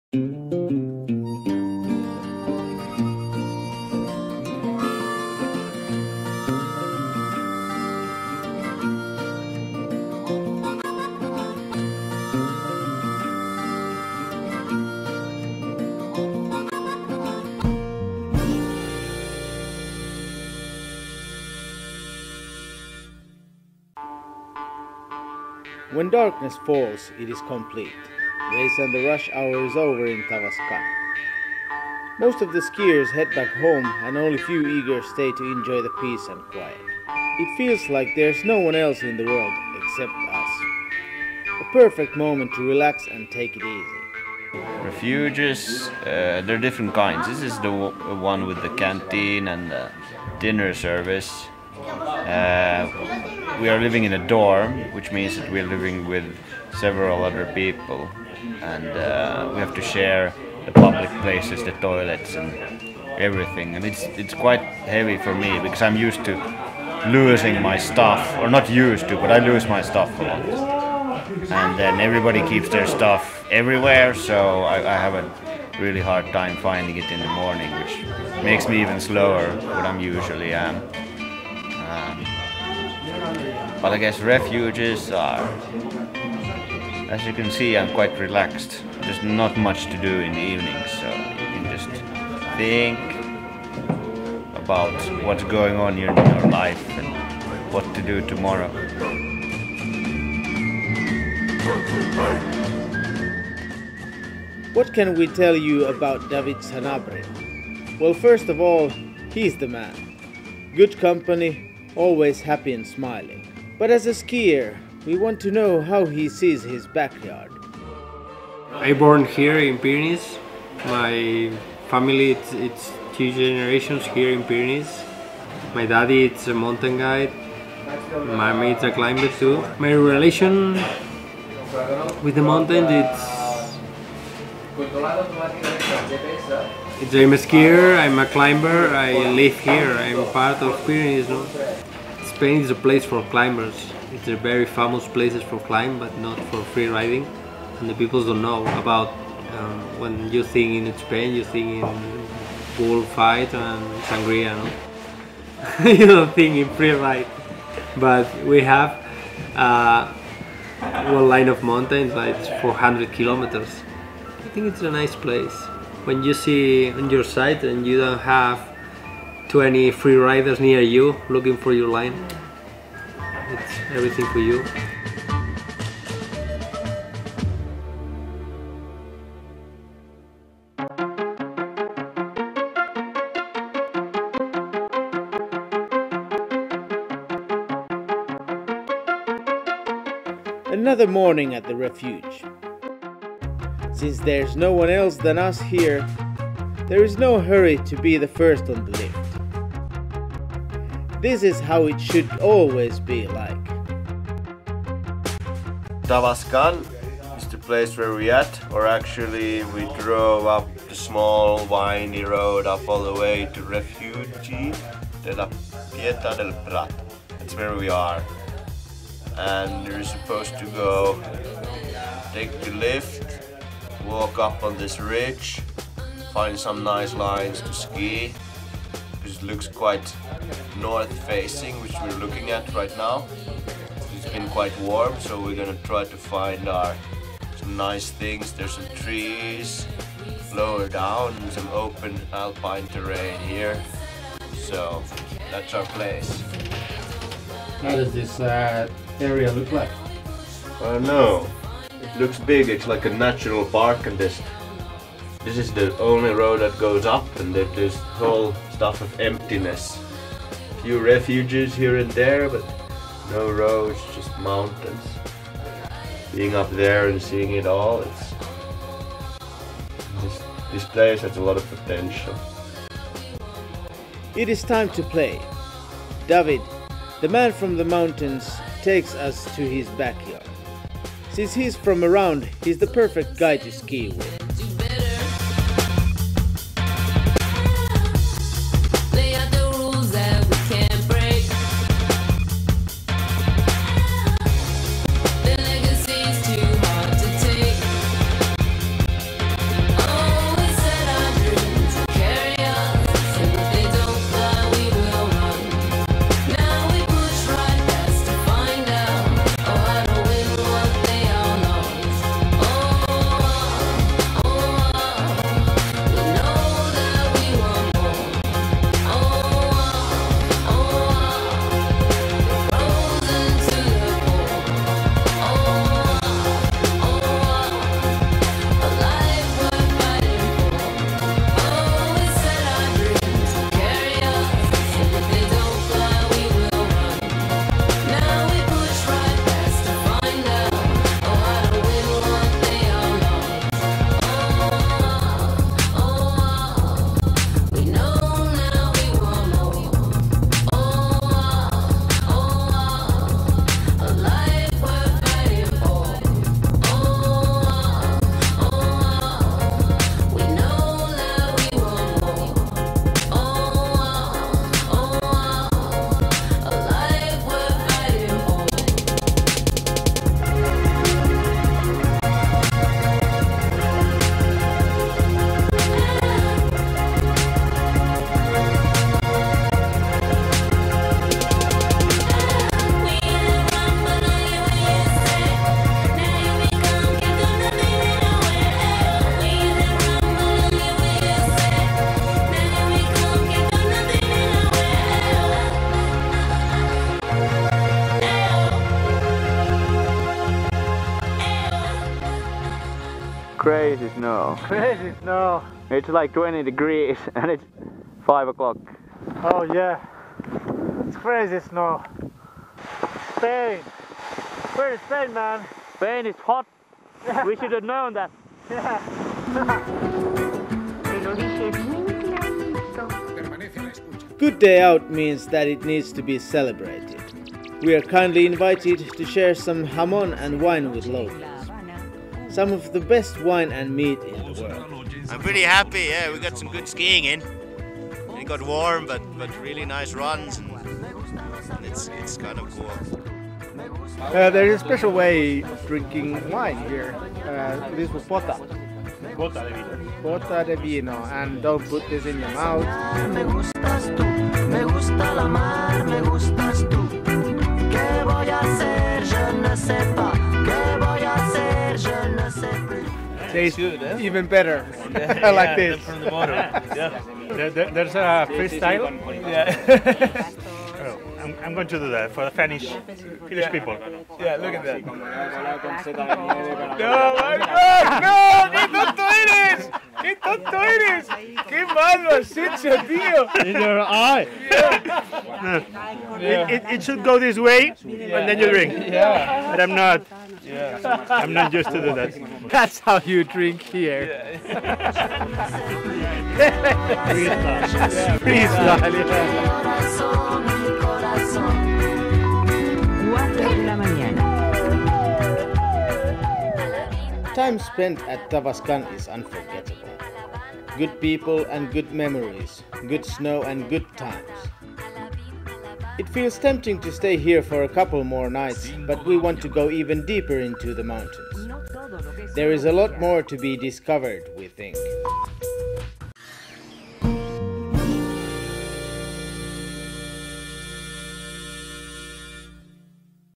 TWS- Finding the Pyrenees part 3/4
Story about finding, experiensing and living the Pyrenees. Good friends, good times and good food accompanied by good snow.

Travellers: Jan-Erik Blomberg and Paul Siljama